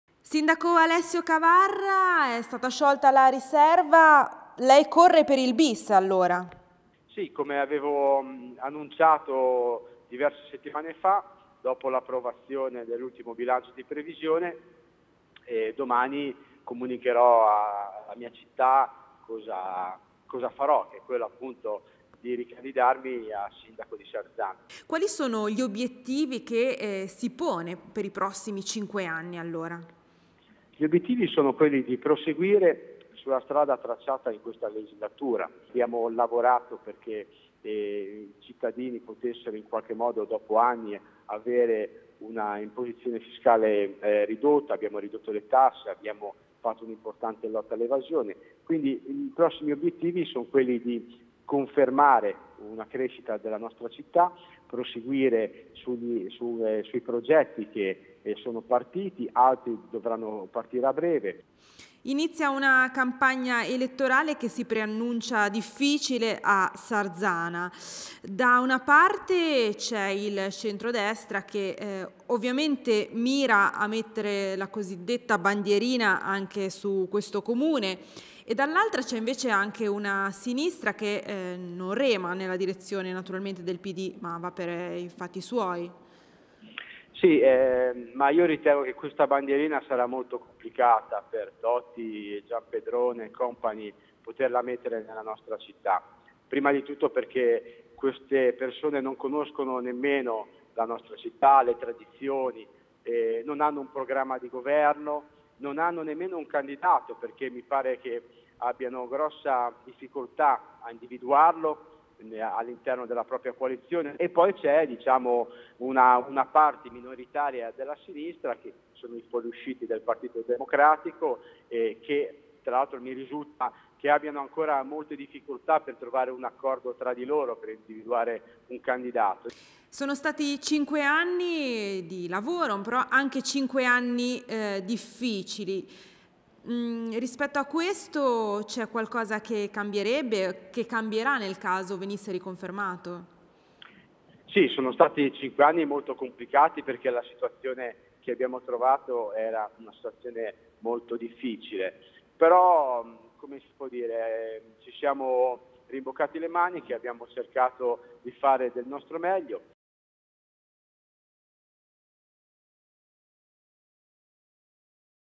Alessio Cavarra candidato a Sindaco di Sarzana col PD 13-01-2018
Alessio Cavarra, sindaco di Sarzana, dopo mesi scioglie la riserva e conferma di correre per il bis. La notizia è diventata ufficiale dopo la riunione dell'unione comunale del Pd di ieri sera che ha confermato la fiducia (due soli astenuti) al primo cittadino. Domattina alle 10.30 al cinema Moderno l'annuncio ufficiale alla città